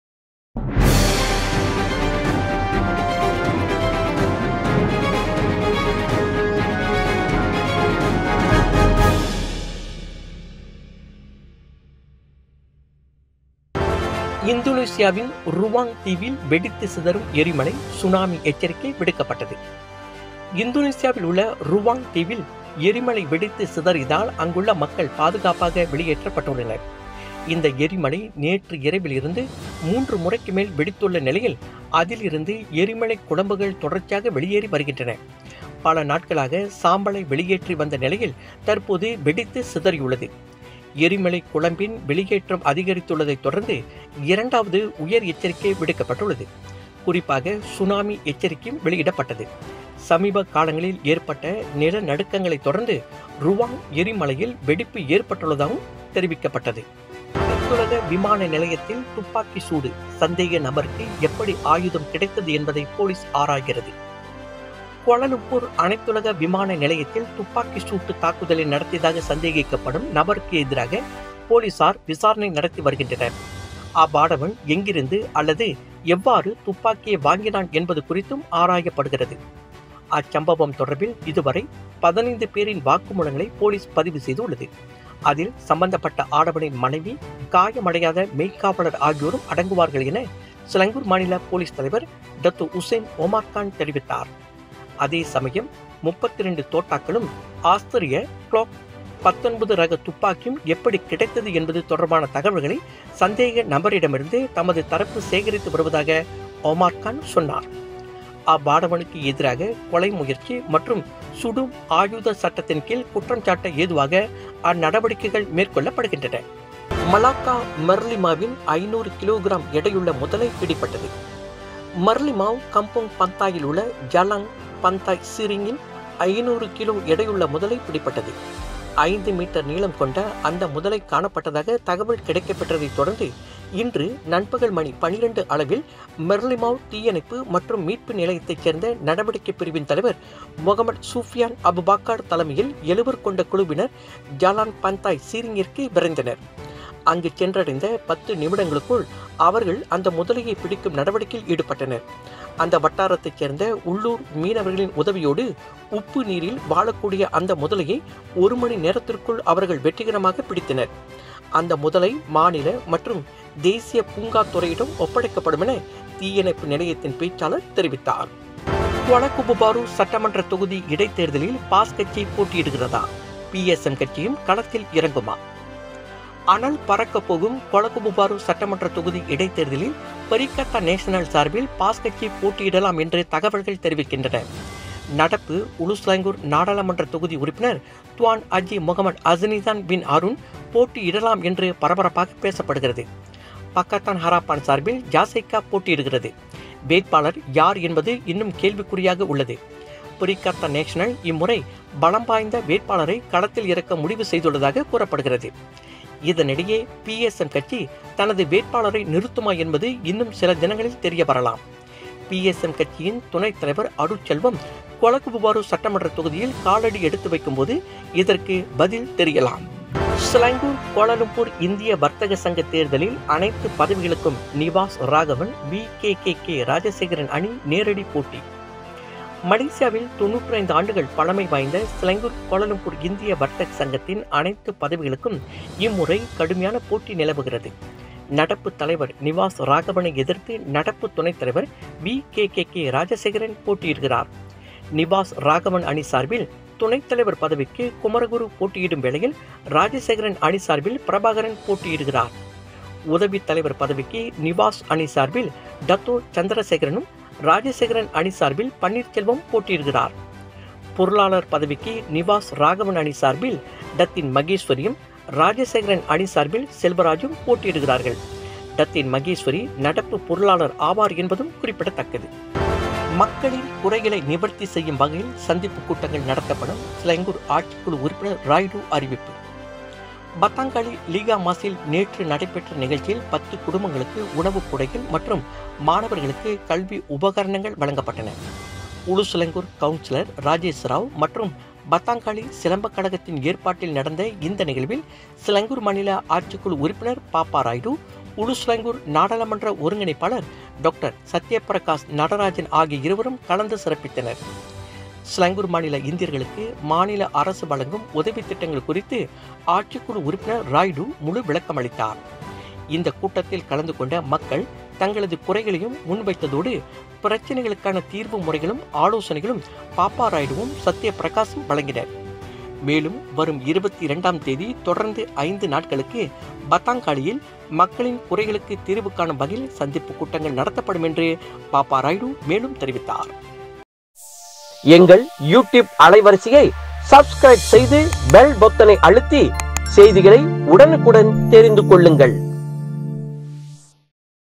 🔴 WATCH NOW | AST News | இந்தோனேசியாவின் ருவாங் தீவில் வெடித்து சிதறும் எரிமலை!
✪  KLIA-வில் துப்பாக்கிச் சூடு ; சந்தேக நபருக்கு எப்படி ஆயுதம் கிடைத்தது? என்பதை போலீஸ் ஆராய்கிறது!

✪  மலாக்கா மெர்லிமாவில் 500 கிலோ கிராம் எடையுள்ள முதலை பிடிபட்டது!

✪ கோலகுபு பாரு சட்டமன்ற தொகுதி இடைத்தேர்தலில்  பாஸ் கட்சி போட்டியிடுகிறதா?
பிஎஸ்எம் கட்சியும் களத்தில் இறங்குமா?